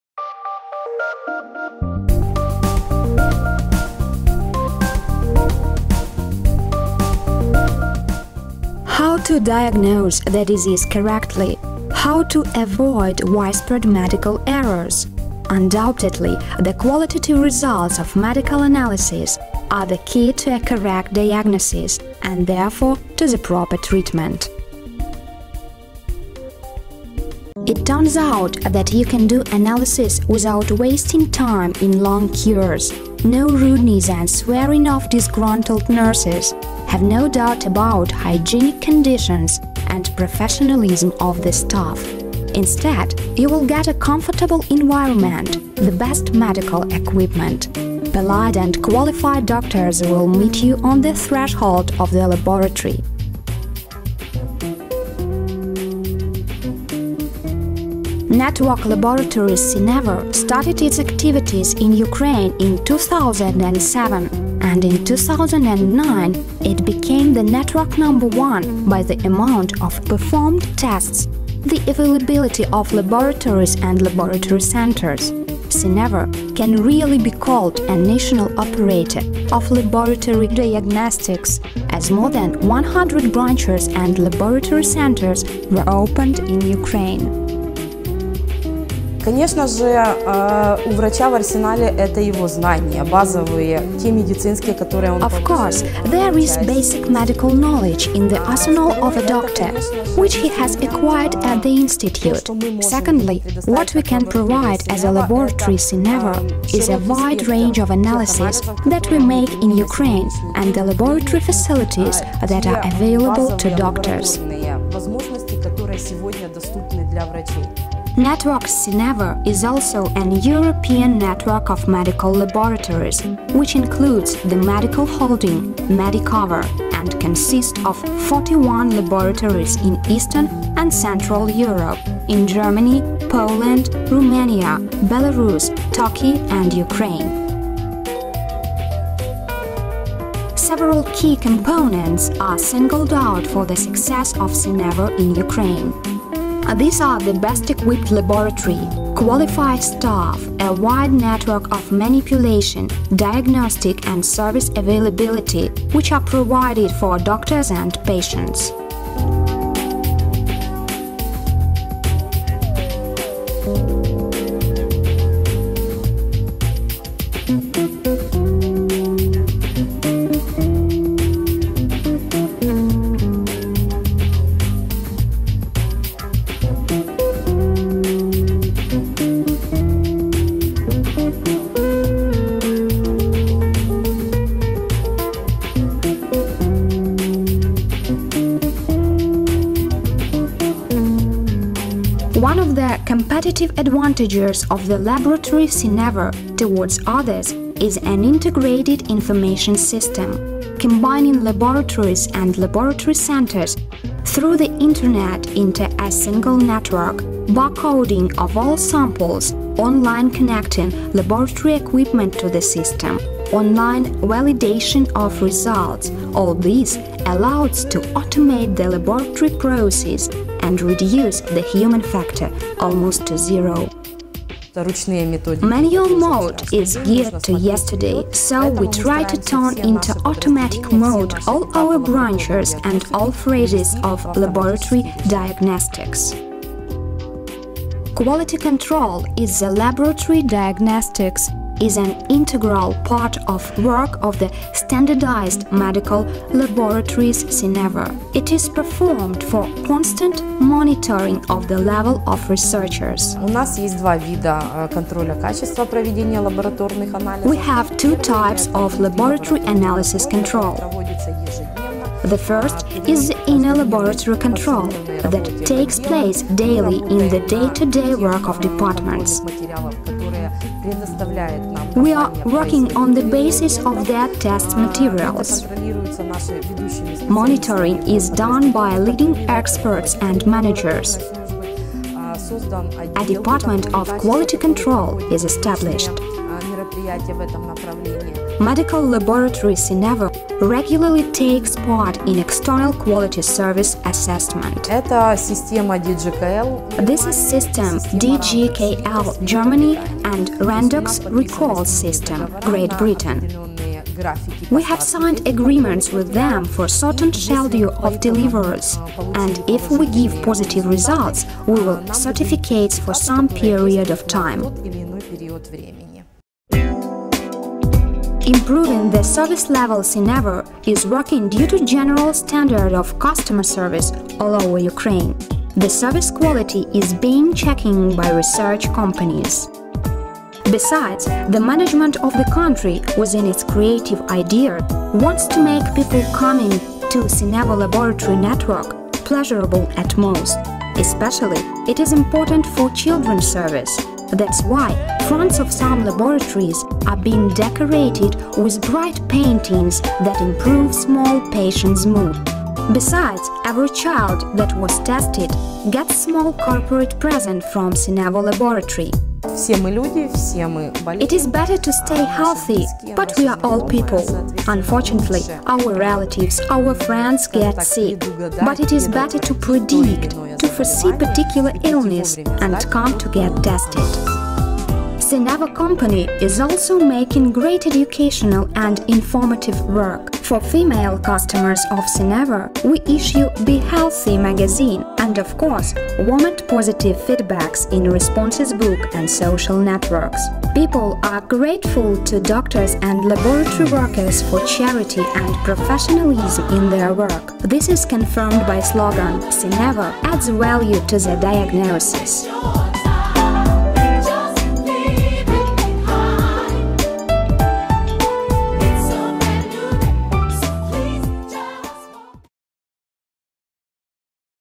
SYNEVO Medical Laboratory (Ukraine)
This video shows the automated working process and modern equipment for laboratory diagnostics, which is using in SYNEVO.